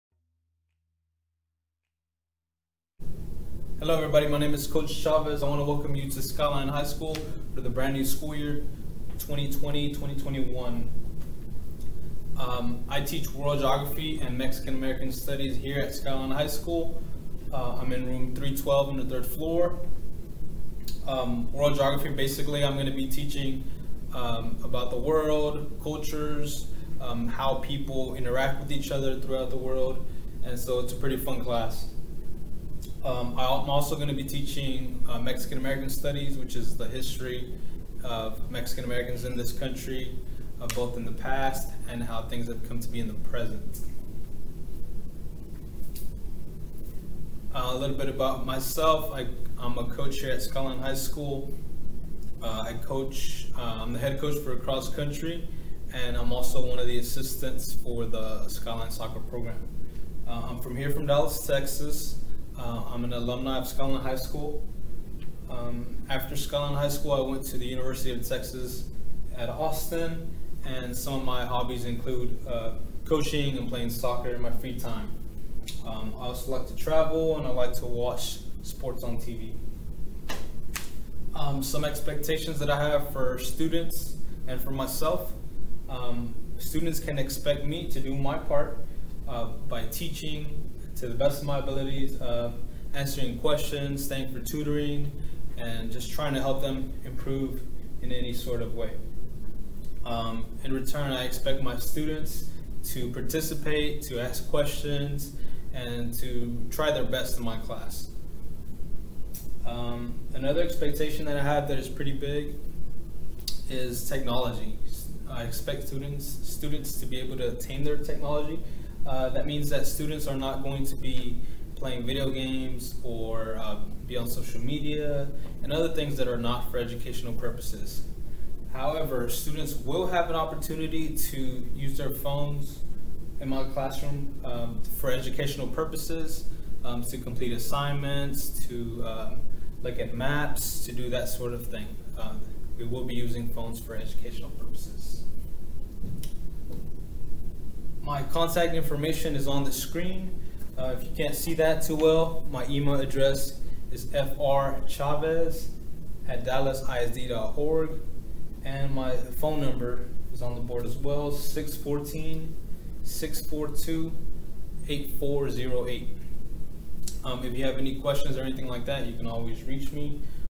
Welcome to Skyline HS!
It's time to begin the 2020/2021 school year at Skyline HS! I look forward to working with you.